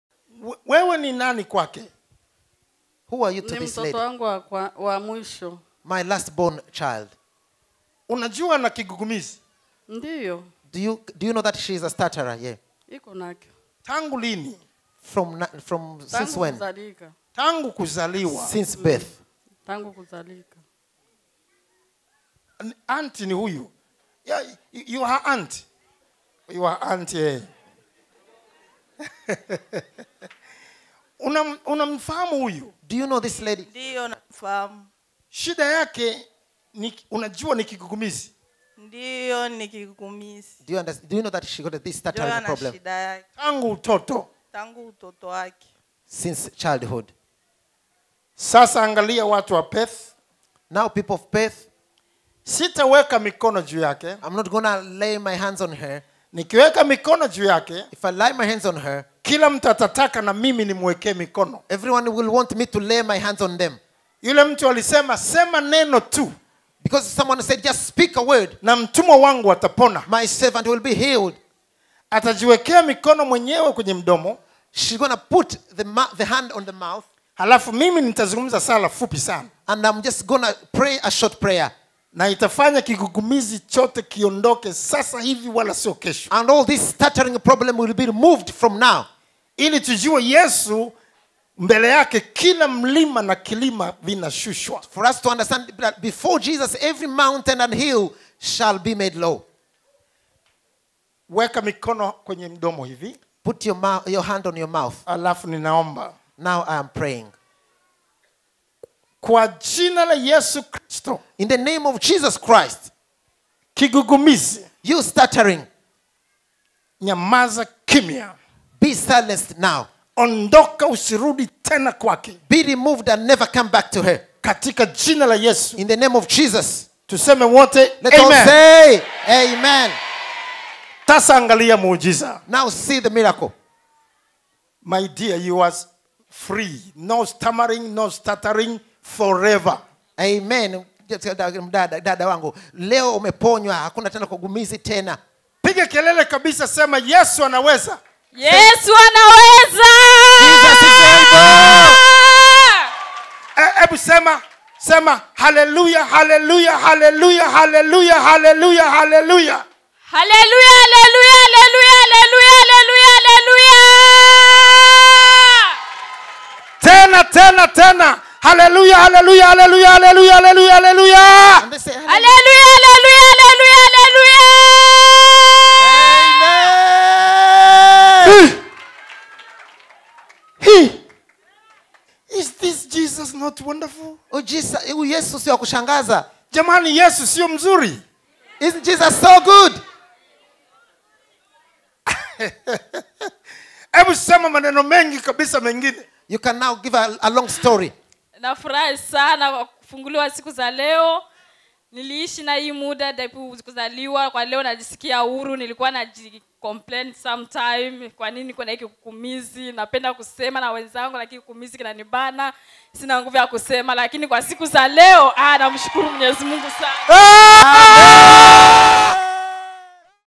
PERTH MIRACLE HEALING CRUSADE, AUSTRALIA 15.5.2016: Stutterer since childhood healed miraculously
Stutterer since childhood healed miraculously at the Perth Miracle Healing Crusade, Australia, on Sunday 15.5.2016